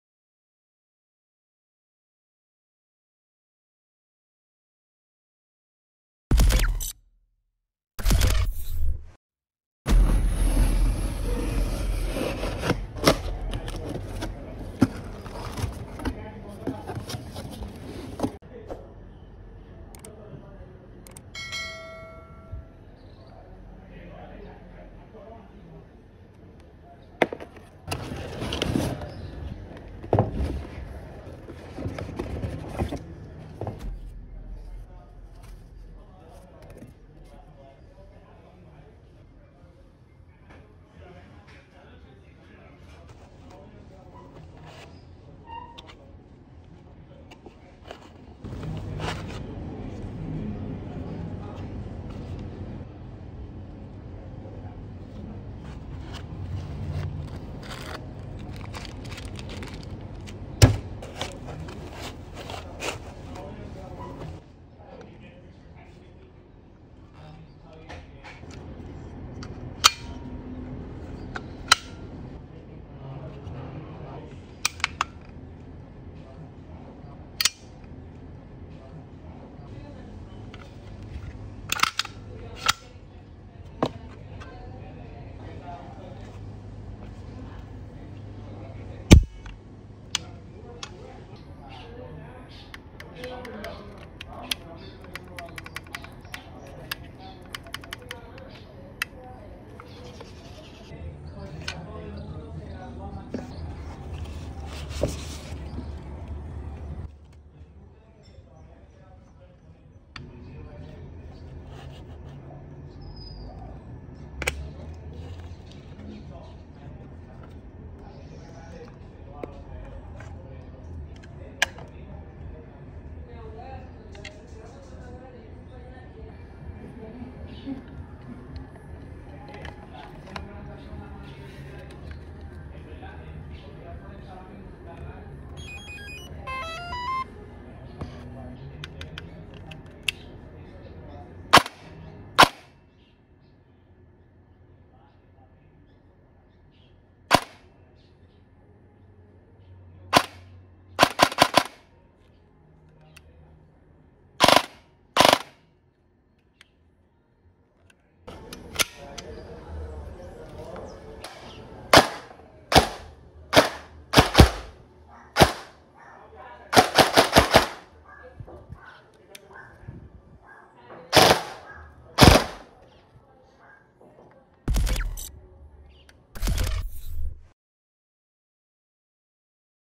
EMG Helios / Airsoft CYMA Platinum DDMK18 Toy Unboxing! (ASMR Unboxing)
Airsoft Toy, not real.
Official Airthor Unboxing of the EMG Helios / CYMA Platinum QBS Daniel Defense MK18 Airsoft AEG.